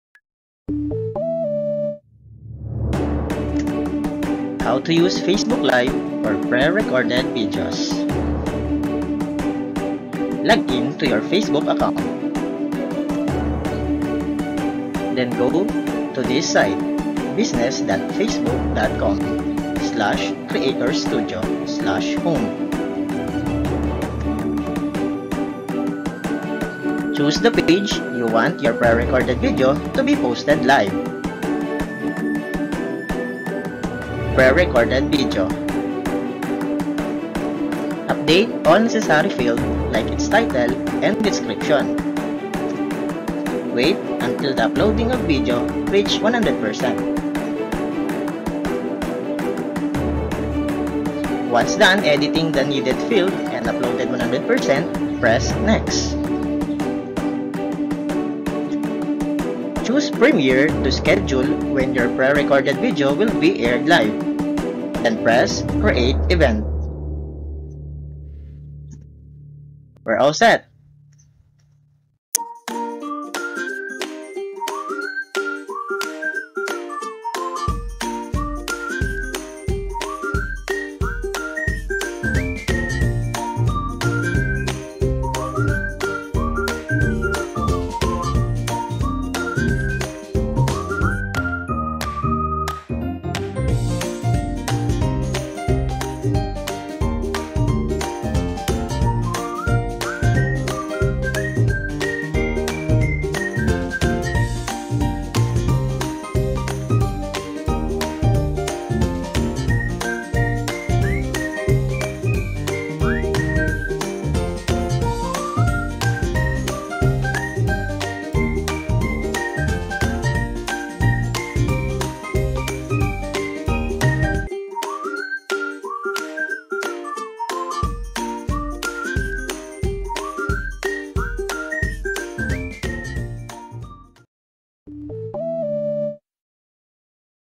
How to Use Facebook Live for Pre recorded Videos
Note: Please be noted that the Pre-recorded video must be uploaded 15minutes to 7days before the scheduled time to be posted LIVE!

1. Log-in to your Facebook Account
3. Choose the page you want your Pre-recorded video to be posted LIVE
4. Upload your Pre-recorded video
5. Update all necessary field like its title, description
6. Wait until the uploading of video reach 100%
7. Choose premiere to schedule when your pre recorded video will be aired live!
8. Choose premiere to schedule when your pre recorded video will be aired live!
9. Then press Create Event
10. Done!